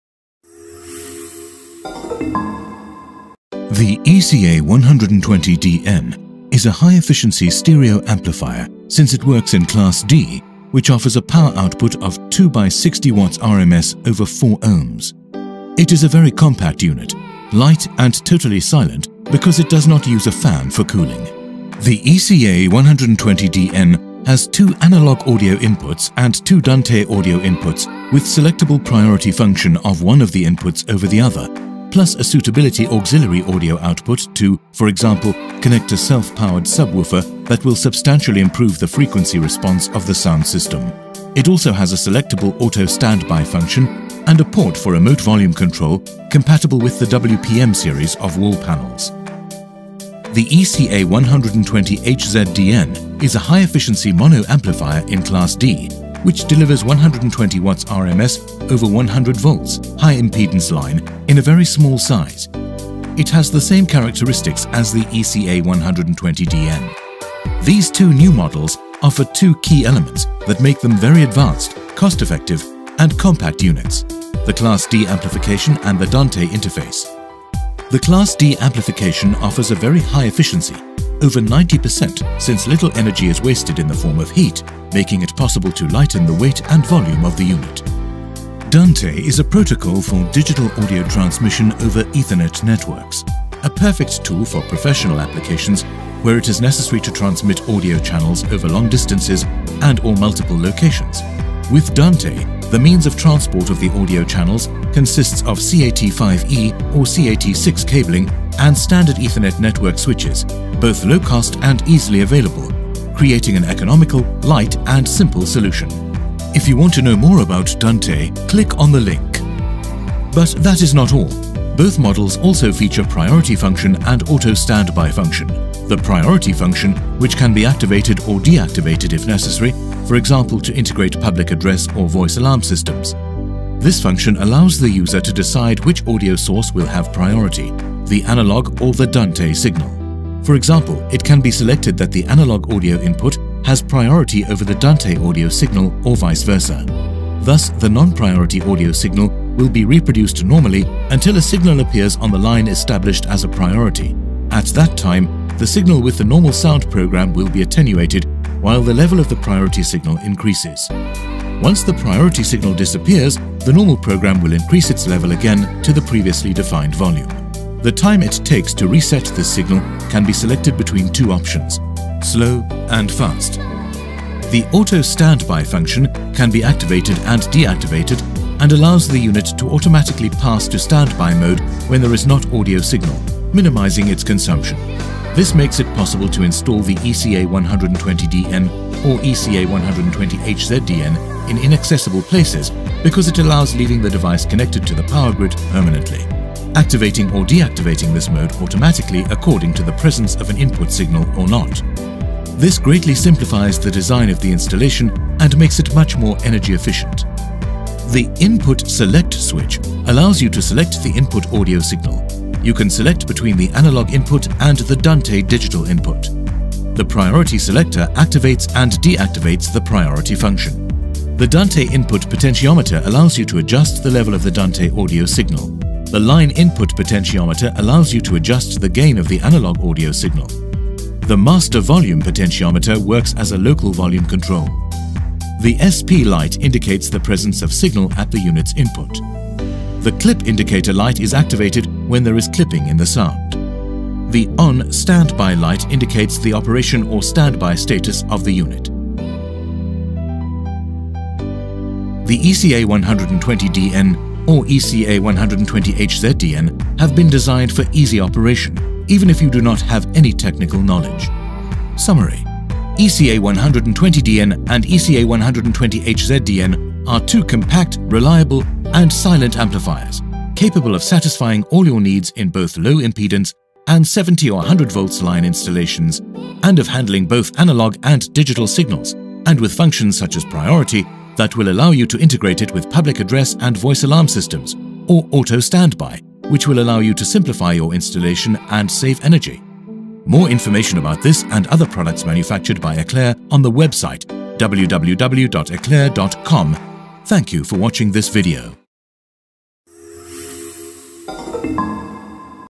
eCA120DN & eCA120HZDN (Long version)
Si desea obtener más información sobre la serie ECLER eCA Dante™, ¡preste atención a este video y tome notas!

(Versión larga)

El eCA120DN es un amplificador estéreo de alta eficiencia ya que funciona en clase D, que ofrece una potencia de salida de 2 x 60WRMS sobre 4Ω. Es una unidad muy compacta, ligera y totalmente silenciosa porque no utiliza ventilador para la refrigeración. El eCA120DN tiene 2 entradas de audio analógico y 2 entradas de audio Dante™ con función de prioridad seleccionable de una de las entradas sobre la otra, además de una salida de audio auxiliar adecuada para conectar un subwoofer autoamplificado que mejorará sustancialmente la respuesta de frecuencia del sistema de sonido. También tiene una función de espera automática seleccionable y un puerto para control de volumen remoto compatible con la serie WPm de paneles de pared.

El eCA120HZDN es un amplificador mono de alta eficiencia en clase D, que ofrece 120WRMS sobre 100V (línea de alta impedancia) en un tamaño muy pequeño. Tiene las mismas características que el eCA120DN.

Estos dos nuevos modelos ofrecen dos elementos clave que los convierten en unidades muy avanzadas, rentables y compactas: la amplificación clase D y la interfaz Dante.